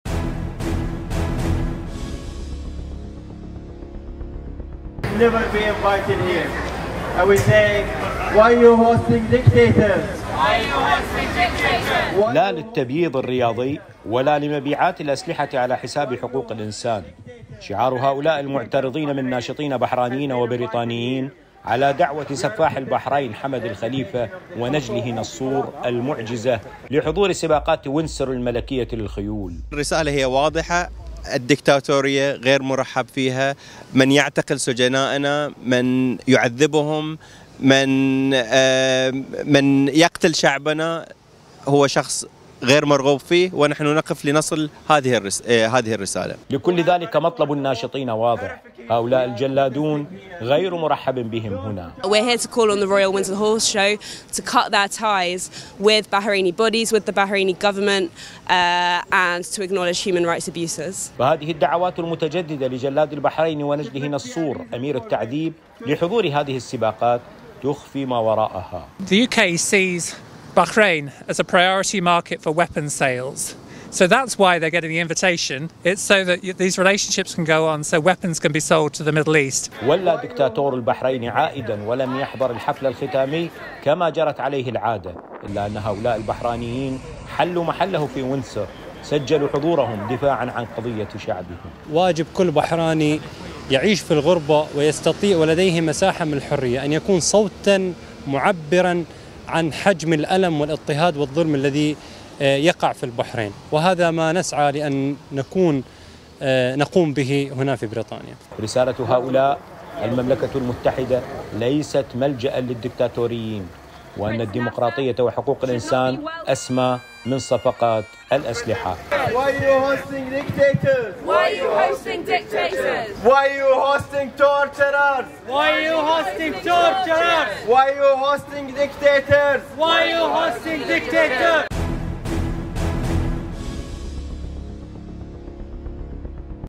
تقرير متلفز: الجلاد حمد الخليفة ونجله أمير التعذيب غير مرحب بهم في المملكة المتحدة
لا للتبييض الرياضي ولا لمبيعات الأسلحة على حساب حقوق الإنسان.
شعار هؤلاء المعترضين من ناشطين بحرانيين وبريطانيين على دعوة سفاح البحرين حمد الخليفة ونجله نصور المعجزة لحضور سباقات ويندسور الملكية للخيول.

لكل ذلك مطلب الناشطين واضح هؤلاء الجلادون غير مرحب بهم هنا.

هذه الدعوات المتجددة لجلاد البحرين ونجله نصور أمير التعذيب لحضور هذه السباقات تخفي ما وراءها.

ولّى دكتاتور البحرين عائدا ولم يحضر الحفل الختامي كما جرت عليه العادة إلا أن هؤلاء البحرانيين  حلو محله في ويندسور سجلوا حضورهم دفاعا عن قضية شعبهم.

رسالة هؤلاء المملكة المتحدة ليست ملجأ للدكتاتوريين وإن الديمقراطية وحقوق الإنسان أسمى من صفقات الأسلحة.